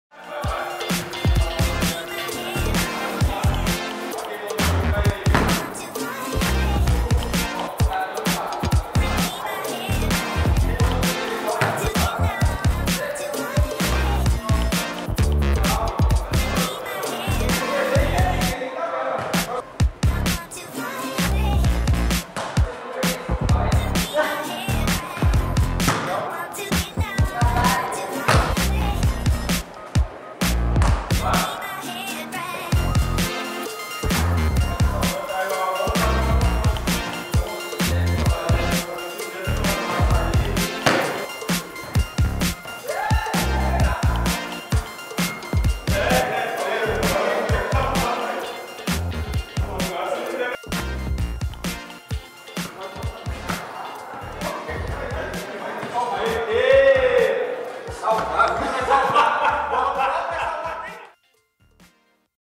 Departamento médico a todo vapor!
Os profissionais do nosso departamento médico trabalham, diariamente, de forma incansável no processo de recuperação dos atletas lesionados. Tudo para que eles voltem o quanto antes aos gramados para dar alegrias ao torcedor!